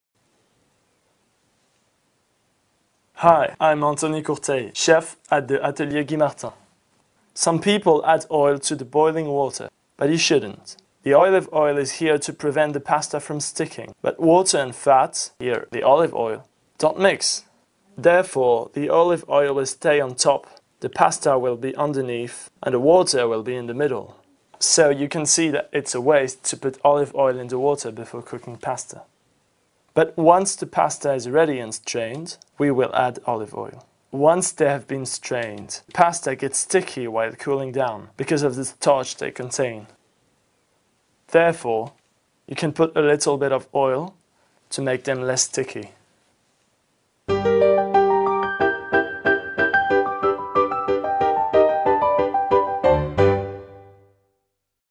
Cooking - Should You Put Oil in the Water Before Cooking Pasta
Anthony Courteille, chef at the Atelier Guy Martin, shows you all the tips to well cook your pasta.
Some people add oil to the boiling water, but you shouldn't put any. Why ? The olive oil is there, so the pasta doesn't stick. But water and fat, here the olive oil, don't mix. So, the olive oil will stay on the top, the pasta underneath, and the water in the middle. So, you can see it is a waste to put olive oil in the water before cooking pasta.
But, once the pasta is ready and strained, we will add olive oil.
Once they are strained, they stick while cooling down, because of the starch they contain. Therefore, you can put a little oil so they stick less.
Anthony Courteille, chef at the Atelir Guy Martin